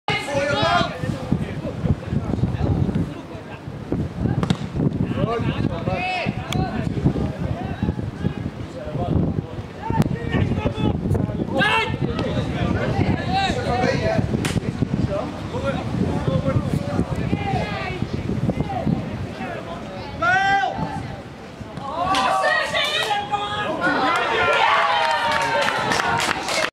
Limmen C1 - Diemen C1 (18-02-2012)
Limmen C1-Diemen C1, 2e goal
Een fragment uit de wedstrijd Limmen C1 tegen Diemen C1 op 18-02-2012.
De wedstrijd werd gespeeld op Dampegheest sportp. - Limmen te LIMMEN en eindigde in een 3 - 1 winst voor Limmen C1.